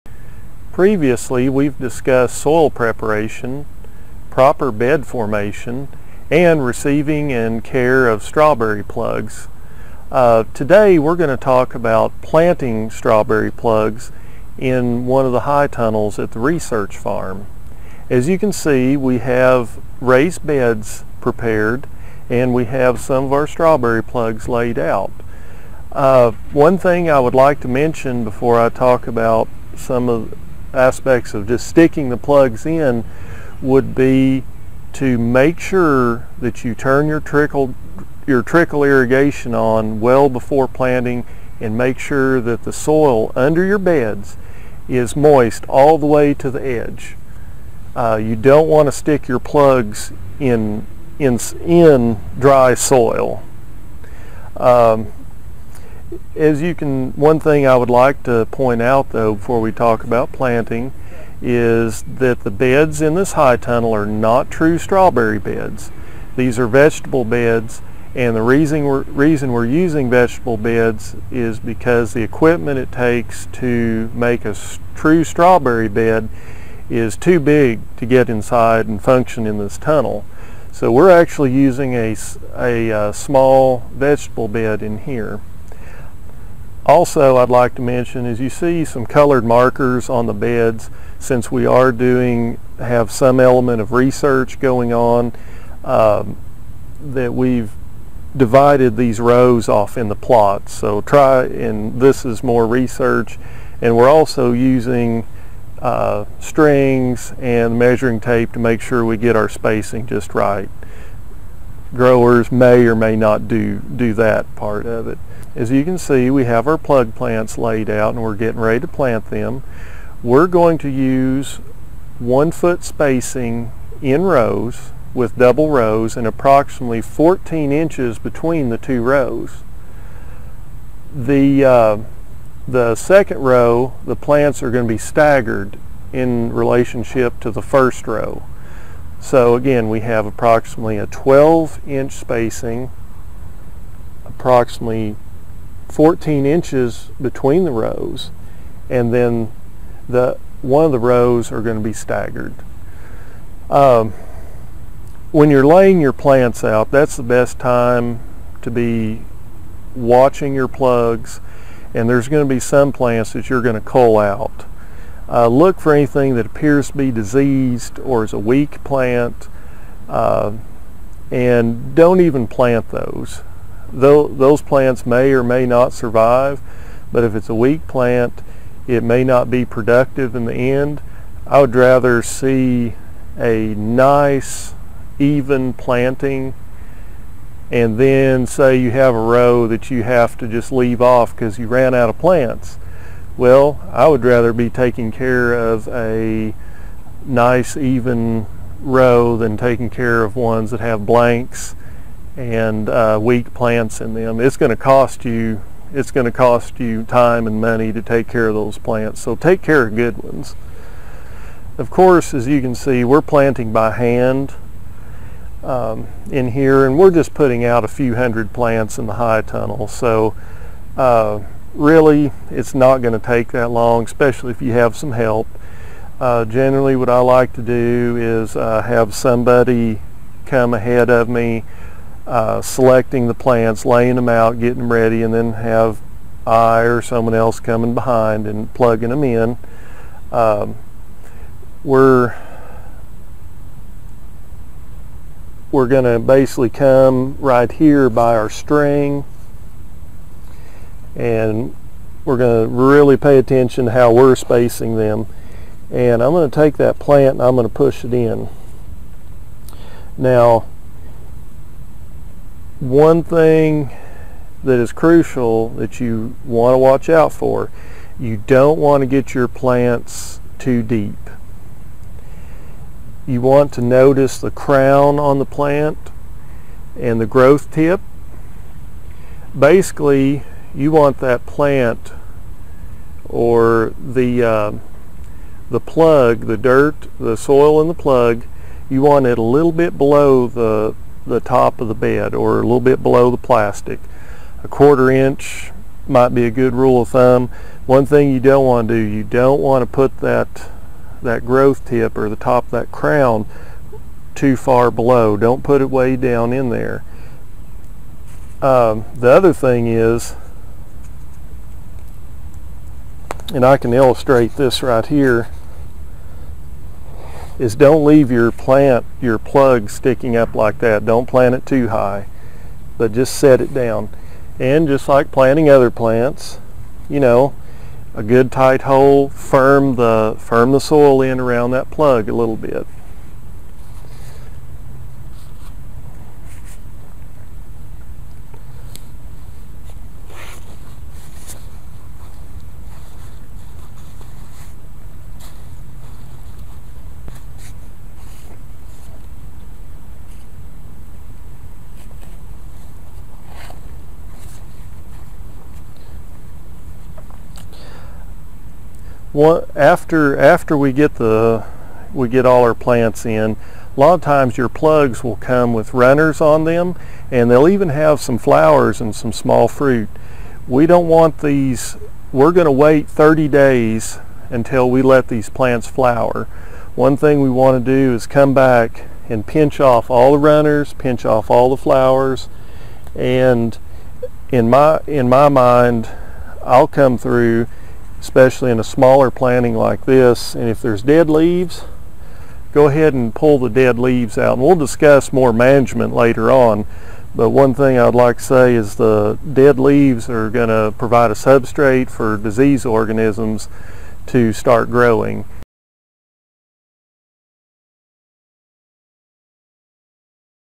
AR Strawberry Production Video 3: Planting Strawberry Plugs in High Tunnels
In this video, viewers will learn how to plant strawberry plugs into raised beds in high tunnels.

This strawberry project is part of the National Strawberry Sustainability Initiative (NSSI), a grants program funded by the Walmart Foundation and administered by the University of Arkansas Division of Agriculture Center for Agricultural and Rural Sustainability.